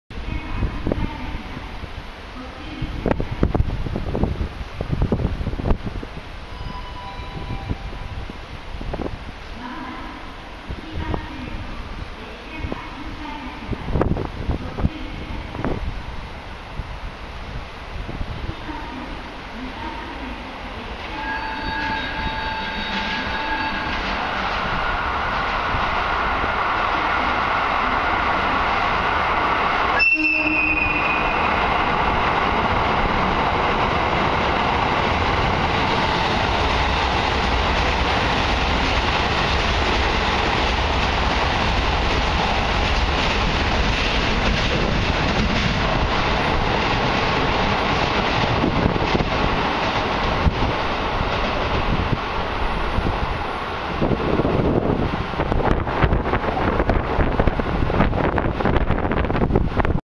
特急オホーツク回送 桑園2012.10.14
２０１２年１０月１４日、札幌駅に到着した特急オホーツク号の編成が車両基地に向かうため函館本線桑園駅を通過する様子です。